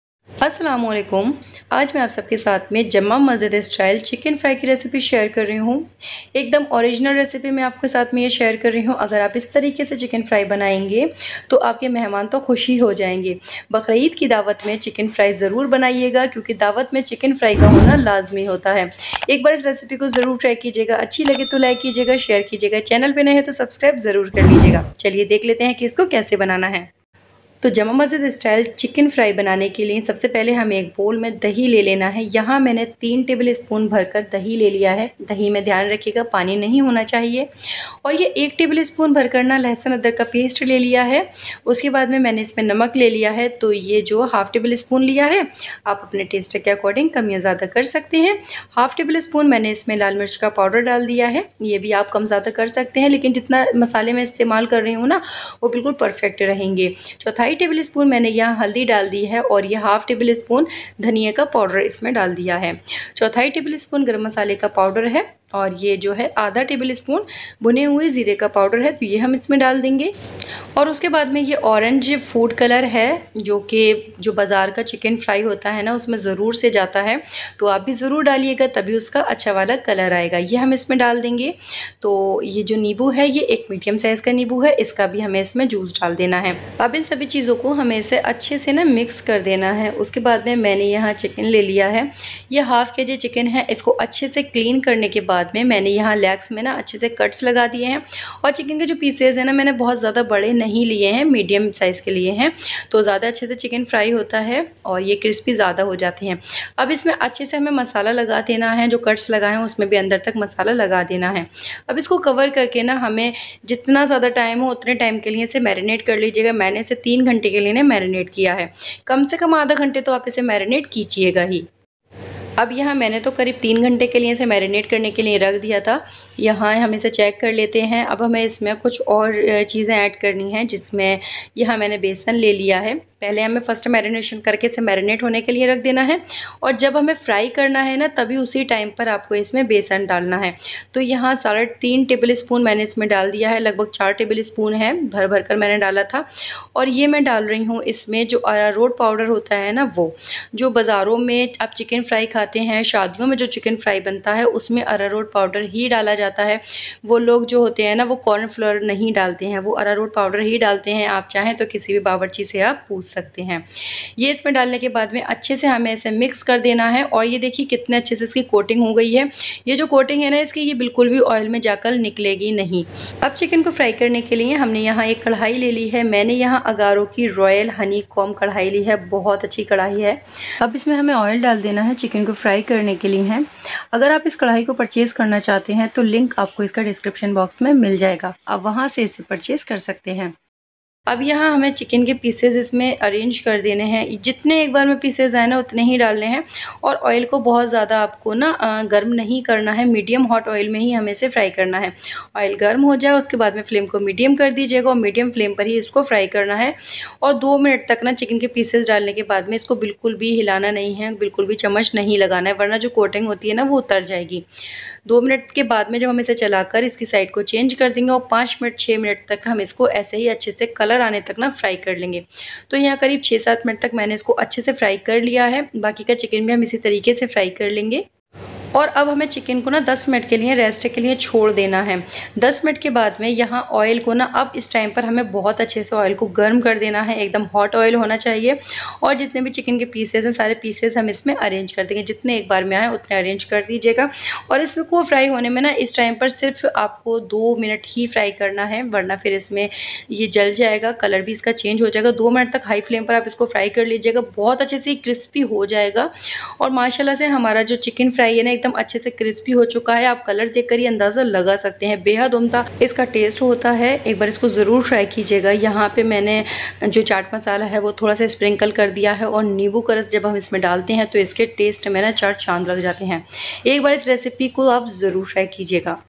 Delhi Jama Masjid Style Crispy Chicken Fry Original Recipe | दिल्ली वाला चिकन फ्राई
LINK TO PURCHASE KADAI :-
AGARO Royal Honey Comb Kadai

INGREDIENTS :-
500 gm chicken
3 tbsp curd
1 tbsp ginger garlic paste
salt to taste
½ tbsp red chilli powder
¼ tbsp turmeric powder
½ tbsp coriander powder
¼ tbsp garam masala powder
½ tbsp roasted cumin powder
¼ tsp orange food colour
1 lemon juice
3½ tbsp gram flour
2½ tbsp arrowroot powder
oil for fry
chaat masala powder
green coriander leaves
lemon juice